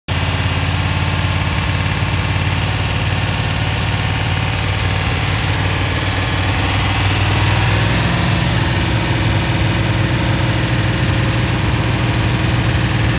1970 Plymouth Barracuda Engine Bench Test
Bench Test of 1970 Plymouth Barracuda 4:15 inch Stroked 440 Engine (nearly 500 cubic inches) professionally built. Edelbrock Performer RPM aluminum heads, TTI headers,